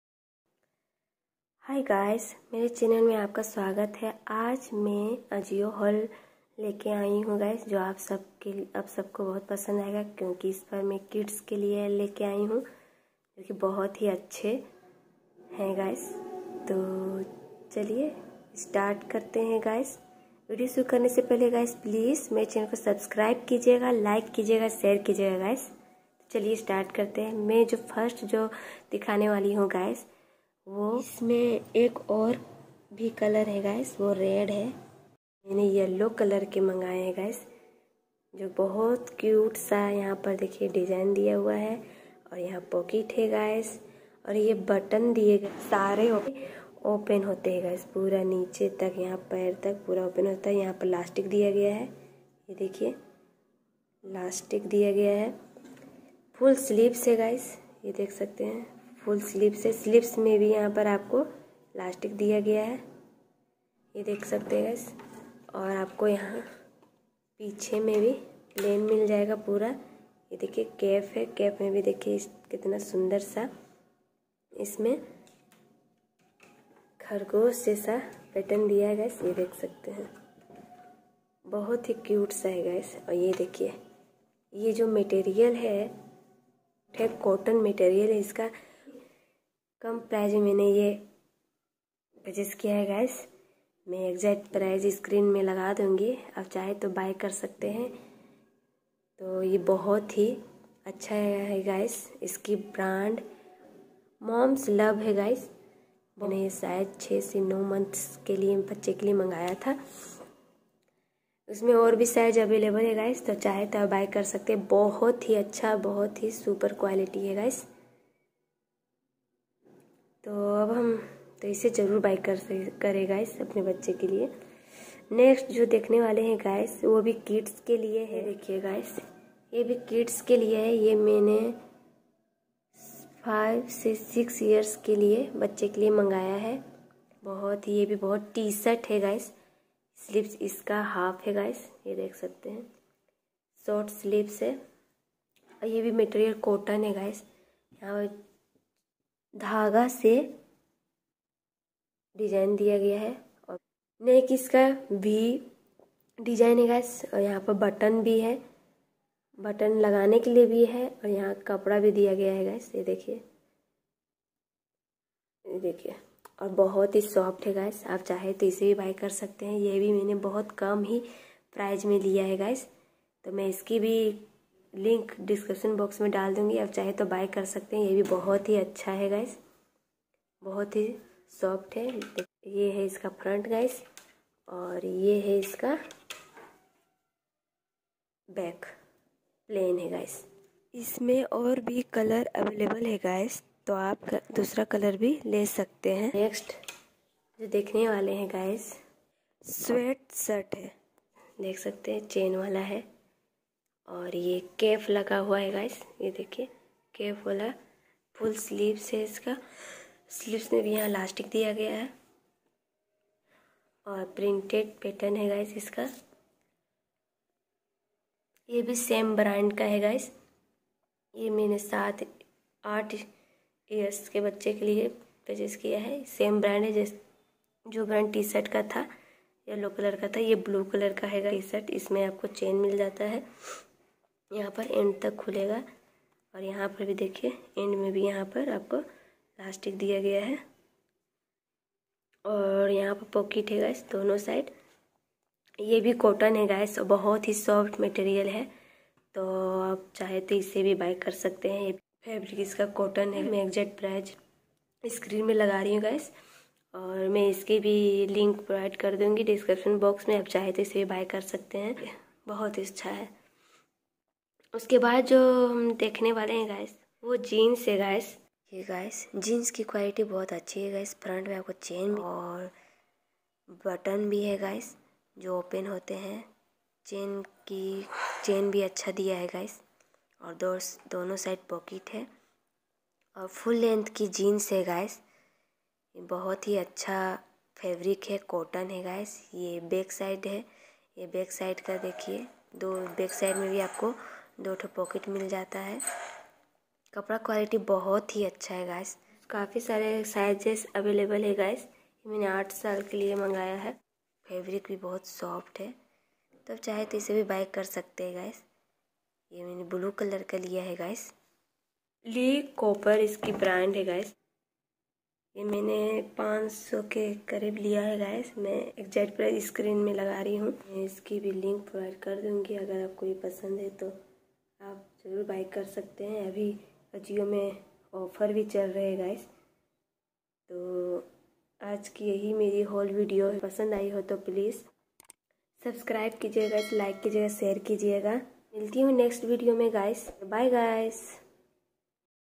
💕🥰latest kids t-shirt 👕 , jeans👖 //Ajio haul /50-90%off/🤗best quality/affordable price🥰💞
Hi 👋 guys

Products link :- 👇

✅ jeans

✅ yellow t-shirt

✅ sweetshirt

✅ Rabbit bottom